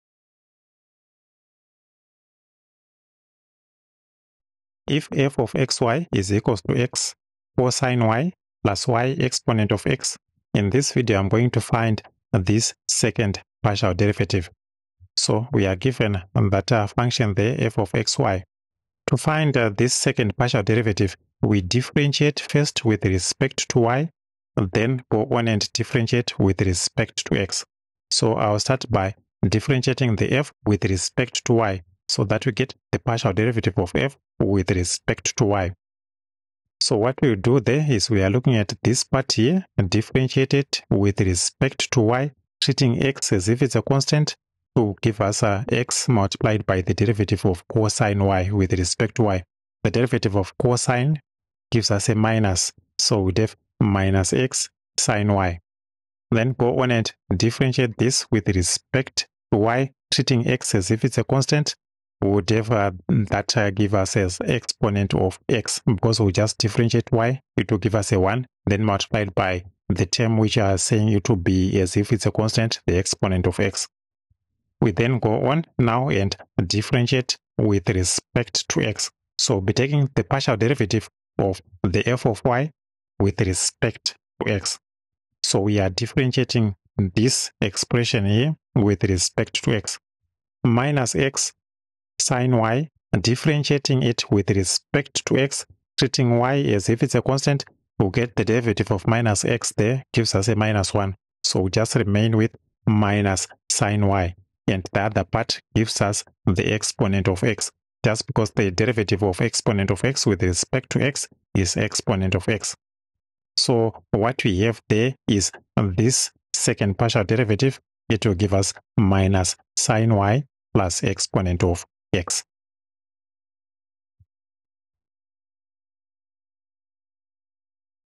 Partial Derivatives.
Evaluating second order partial derivatives.